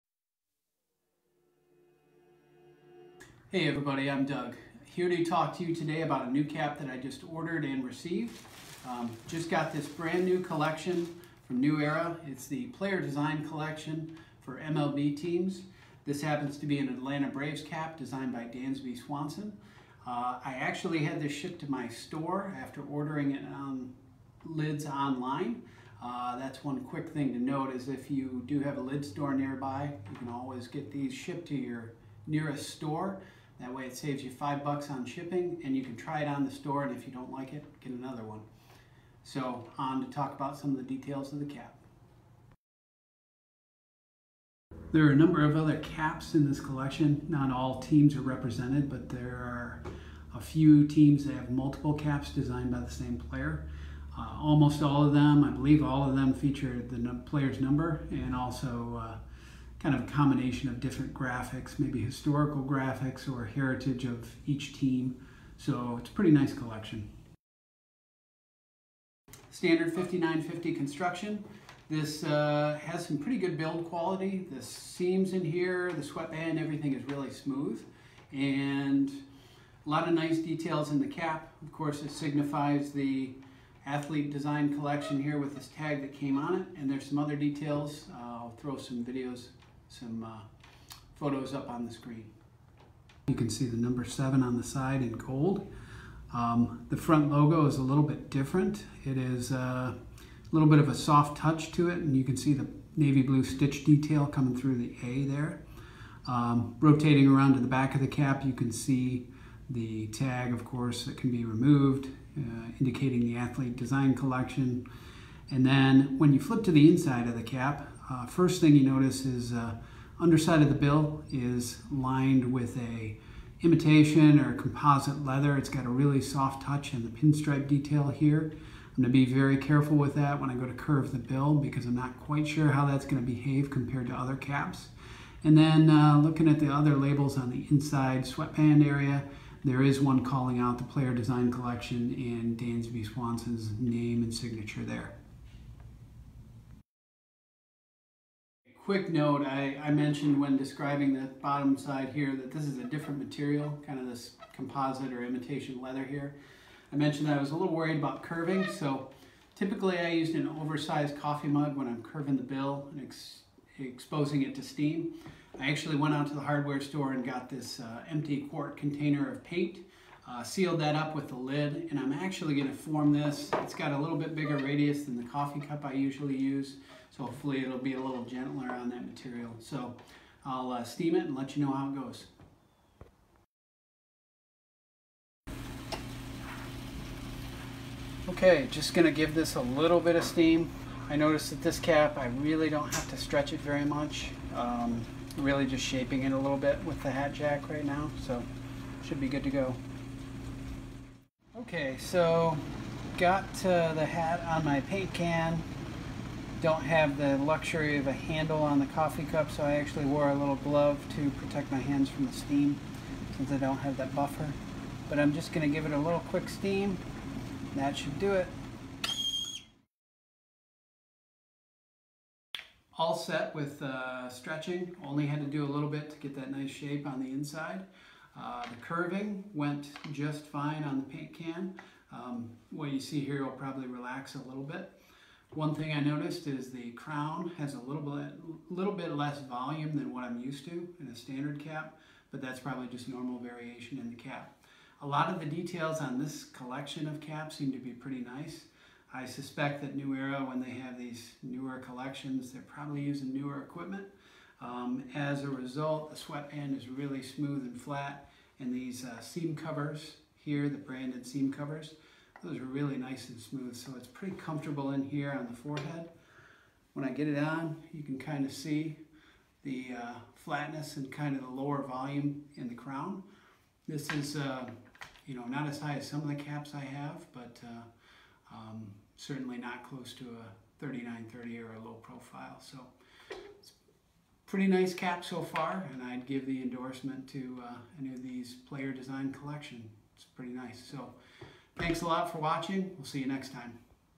Cap Shaping+Review - 59Fifty Player Designed Collection
Overview of New Era MLB Player Designed caps, and a review of one Atlanta Braves/Dansby Swanson cap.  Also shape and curve the hat for a comfortable fit.